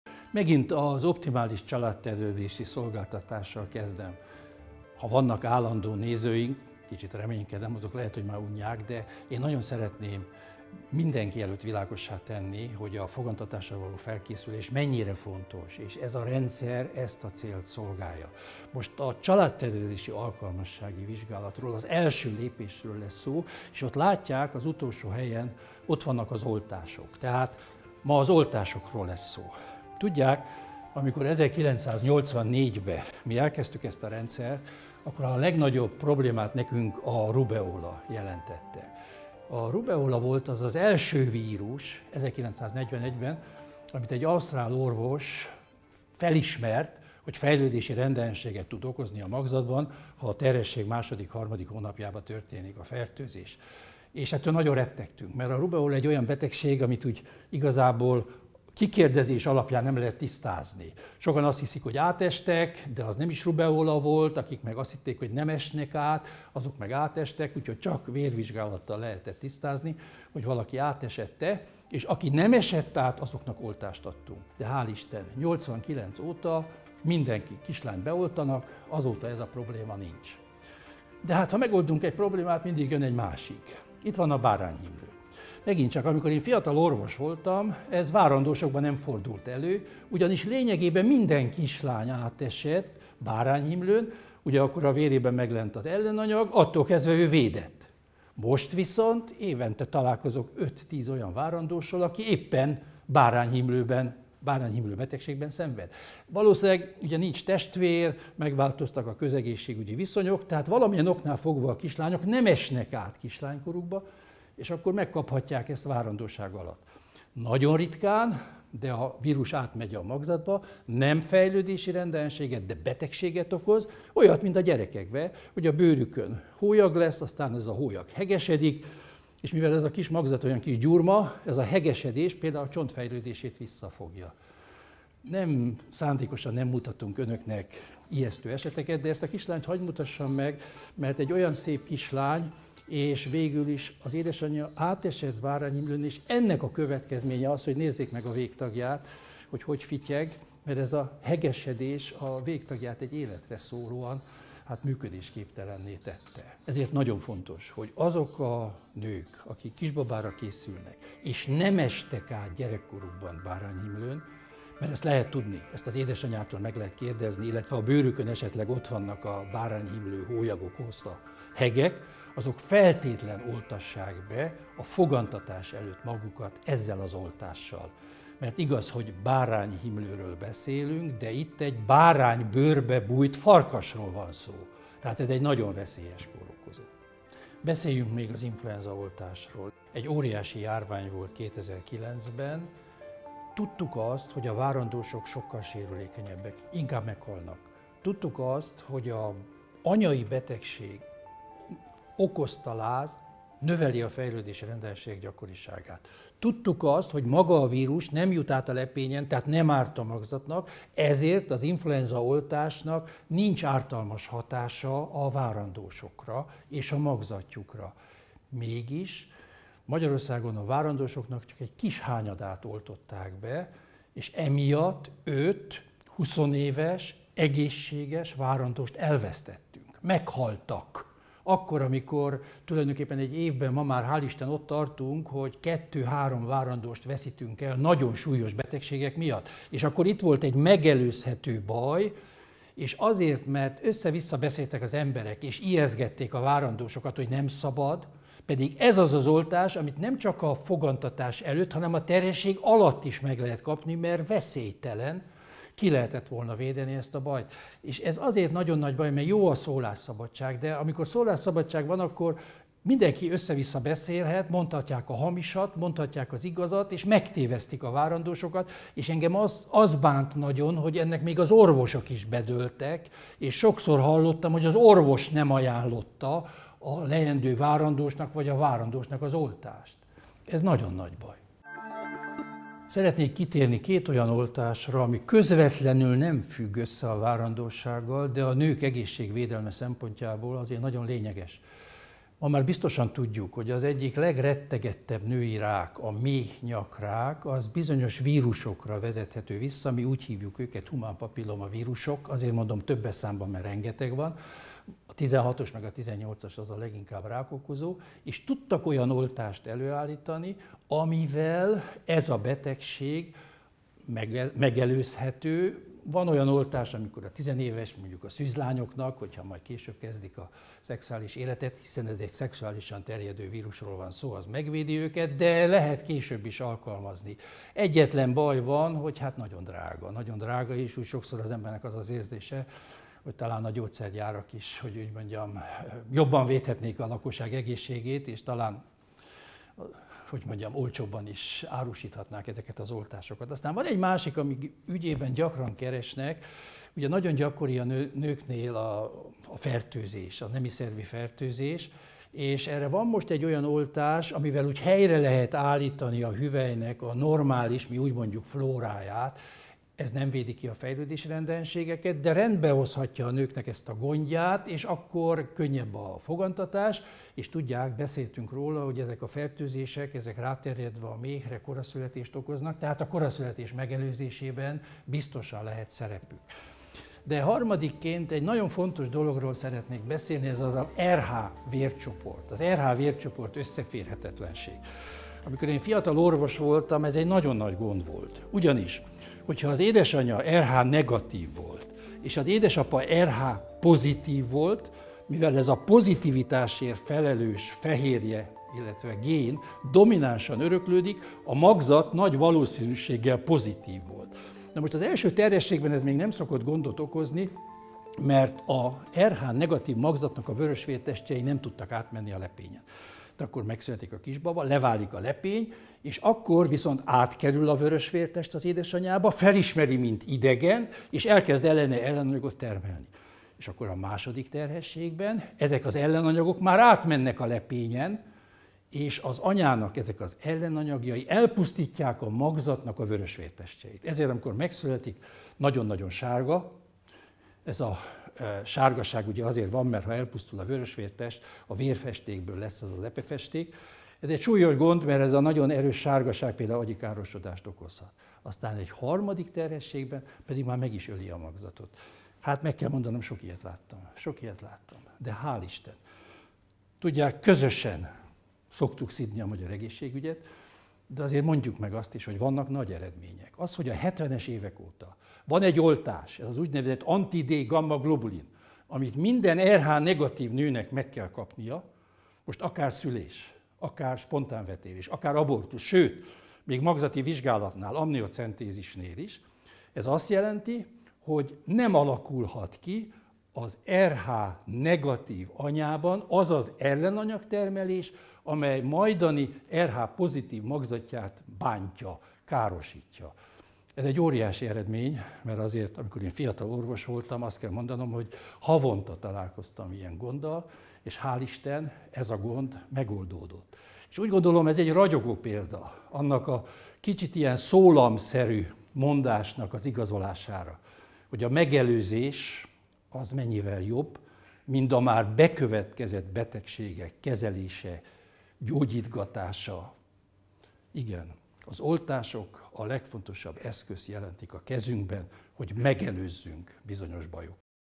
Oltások
Az oltásoknak köszönhetően sikerült az emberek tömeges halálozását okozó nagy járványokat megfékezni. A rubeola, bárányhimlő és influenza elleni oltás nemcsak a várandósok életét védi, de magzatjaikban az e kórokozók által okozott rendellenességek megelőzésére is alkalmas. Nagy baj, hogy az ártalmas mende-mondák miatt sokan nem élnek a megelőzésnek ezzel a hatékony eszközével. (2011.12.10.) A műsor létrejöttét a Sejtbank őssejtbank, a hazai őssejt megőrzés európai színvonalú szolgáltatója támogatta. A Sejtbank a Cryo-Save csoport tagja.